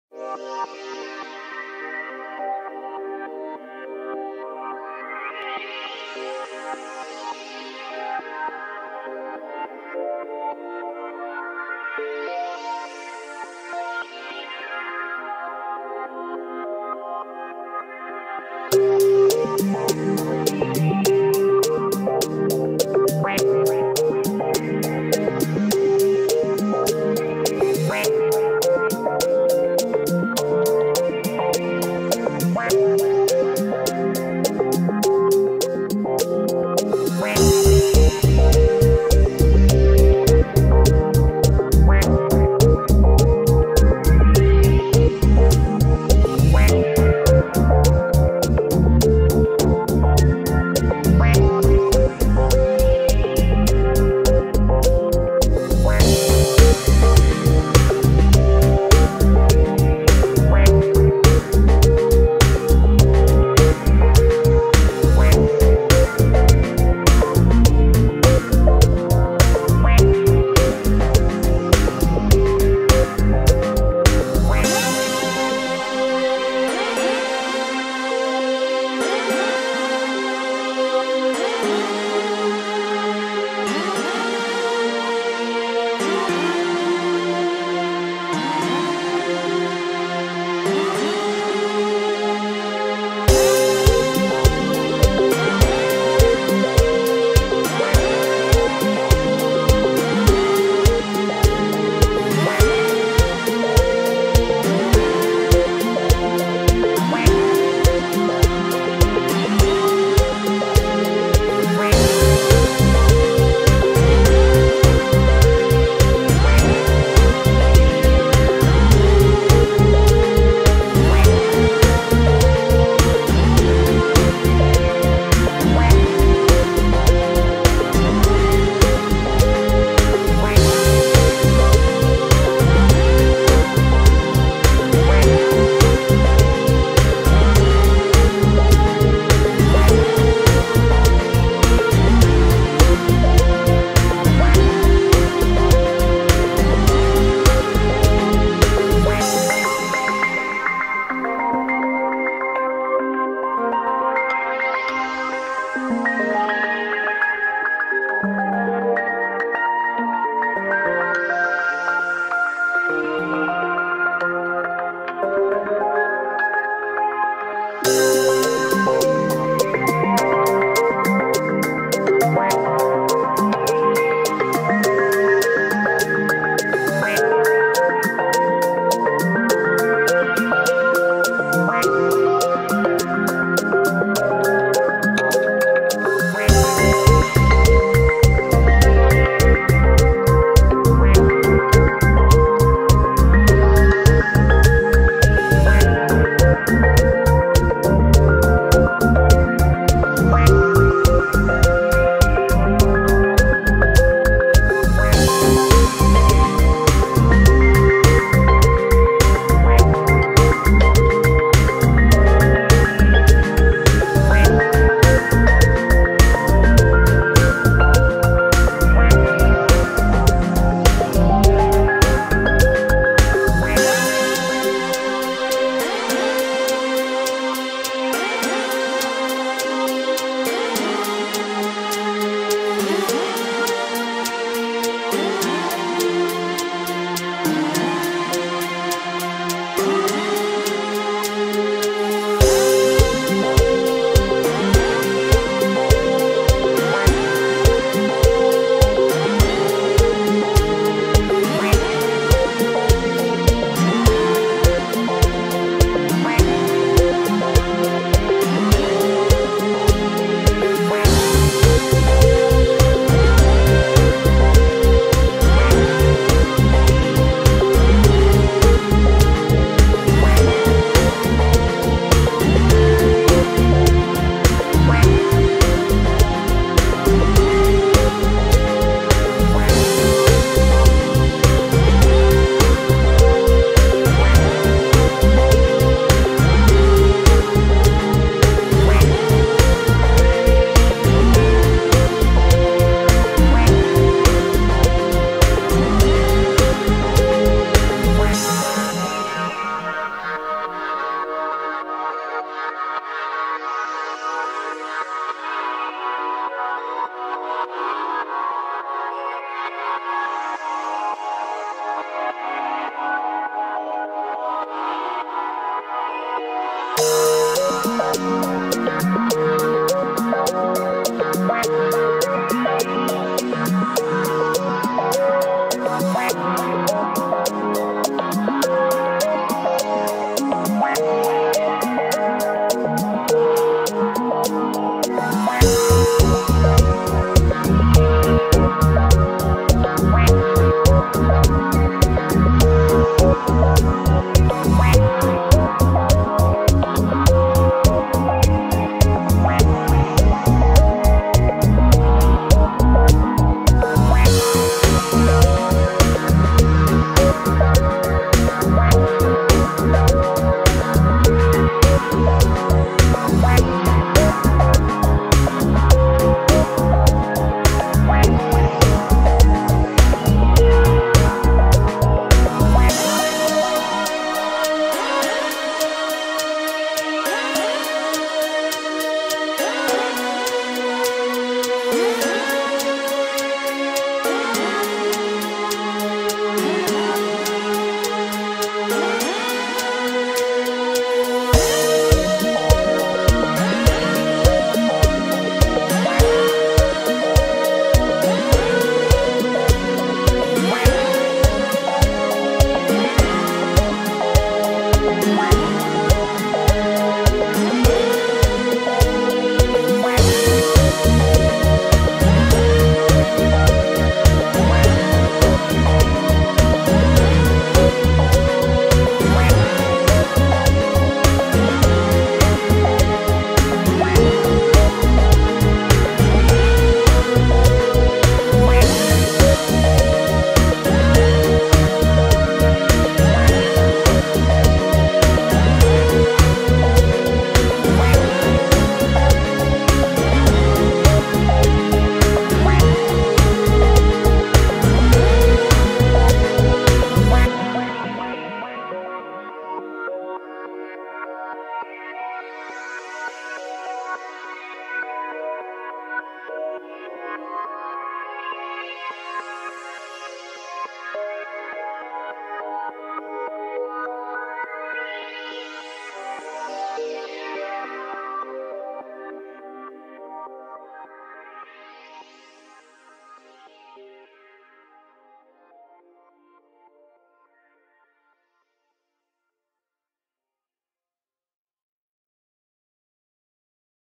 Мощная и Спокойная Красивая Музыка Саванны!
Гепарды! Эти невероятно красивые и элегантные кошки саванны! Им посвятил этот трек..!